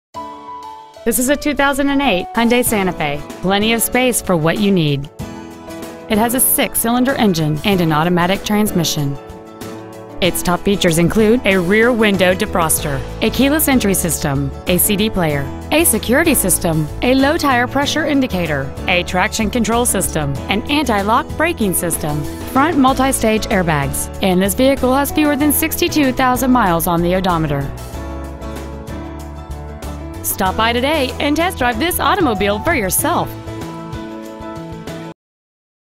2008 Hyundai Santa Fe Plano TX
Year : 2008

 Make : Hyundai

 Trans . : Automatic

 Exterior : Platinum Sage

 Miles : 61,375

 Interior : Gray

 Ford Country

 786 Valley Ridge Cir.

 Preowned 2008 Hyundai Santa Fe Lewisville TX